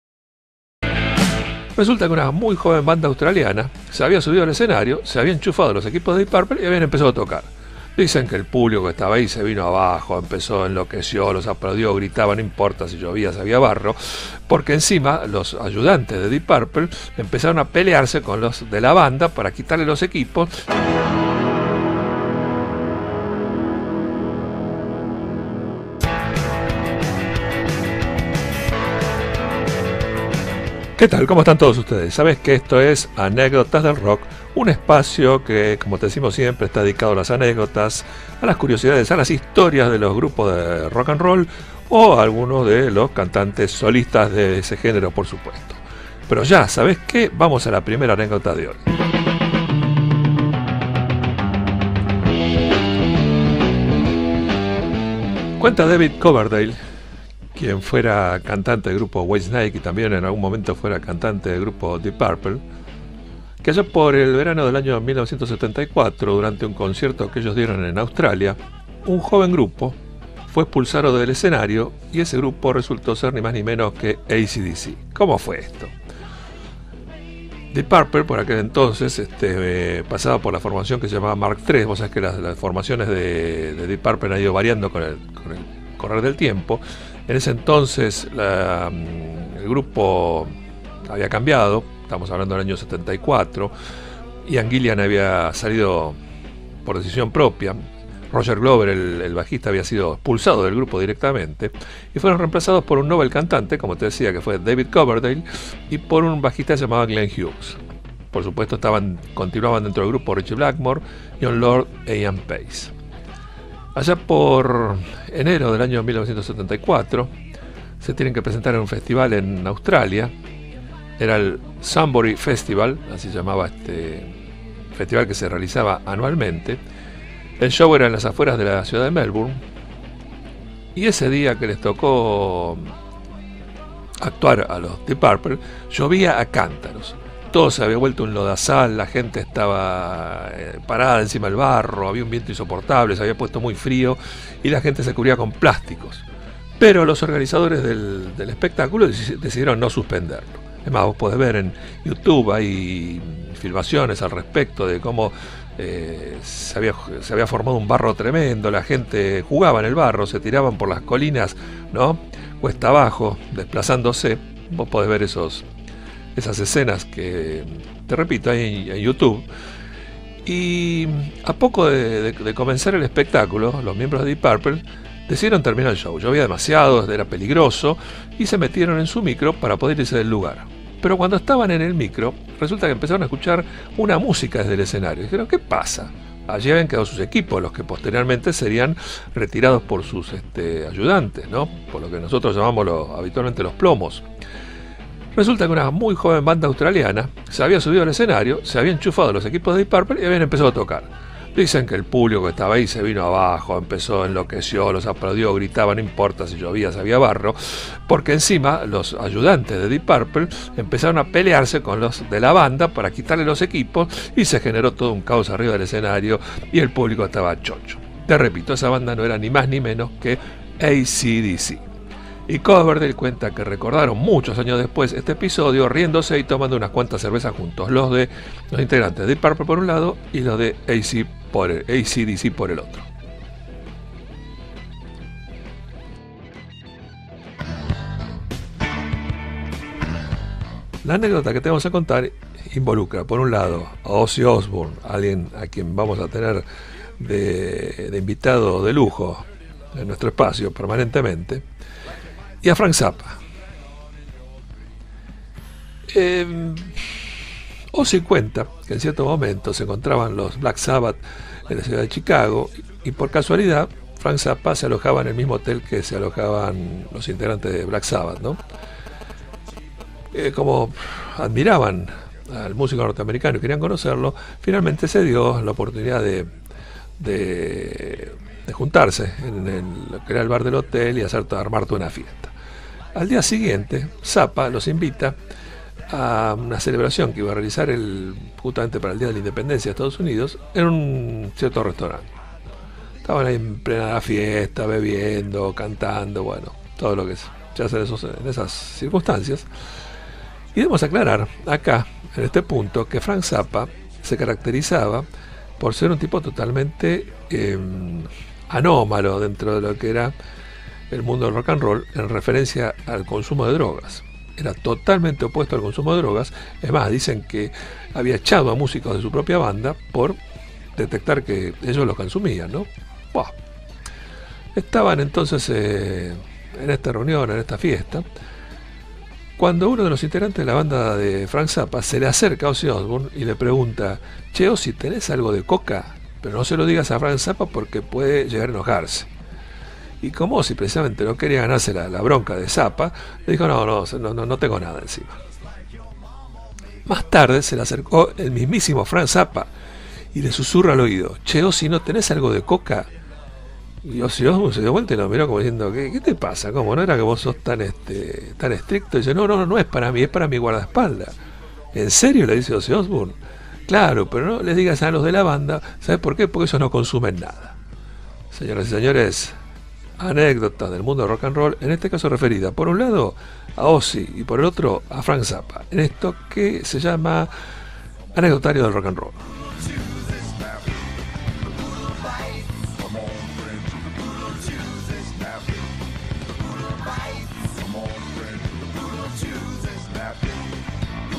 ACDC le quitó los equipos a Deep Purple. El encuentro Ozzy Osbourne y Frank Zappa
Otra vez Anécdotas del Rock. En esta ocasión de cuando los AC/DC tomaron "prestados" los equipos de Deep Purple  y se armó un escándalo. La segunda anécdota involucra a Ozzy Osbourne y a Frank Zappa y un extraño pedido del músico norteamericano.